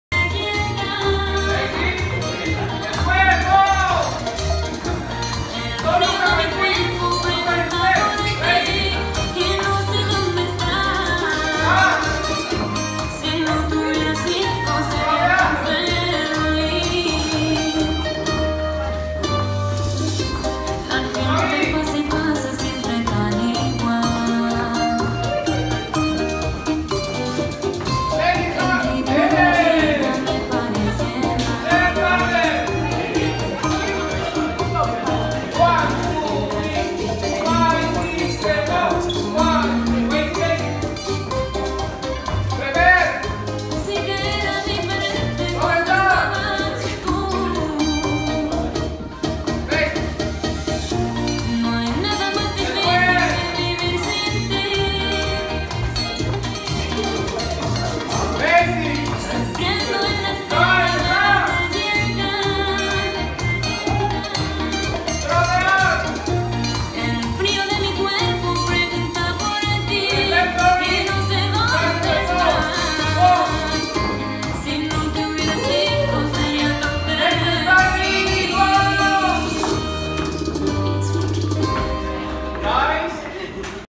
Latín Club Dance School August 12th Social Dance
My beginners' class ! I love to see their evolution and their smiles during the class !